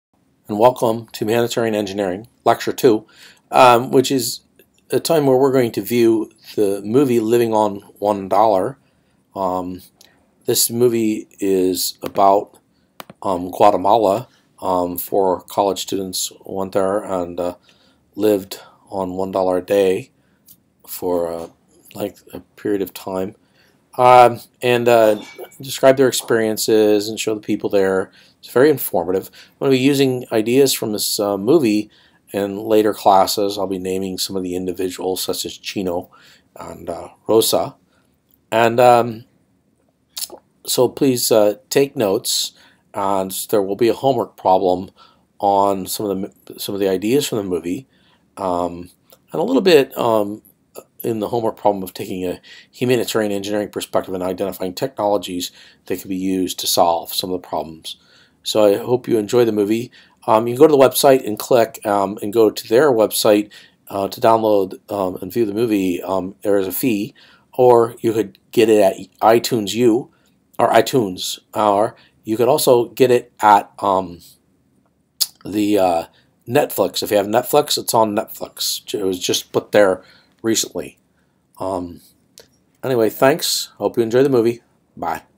Humanitarian Engineering, Lecture 2
These lectures are based on a course, ENGR 5050 Humanitarian Engineering, at The Ohio State University by Prof. Kevin M. Passino, .

Course summary:
Poverty, sustainability, culture, social justice, development strategies, engineering for sustainable community development. Analytical methods (e.g., community dynamics, use of Matlab).